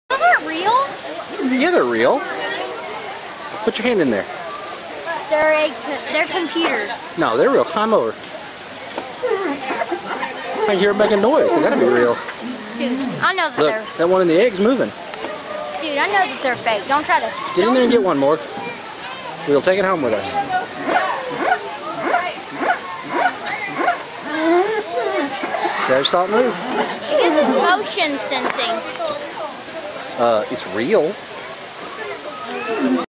Fact or Faked? Dinosaurs.
A candid discussion about whether or not the fake dinosaurs at the Ft. Worth Zoo are real.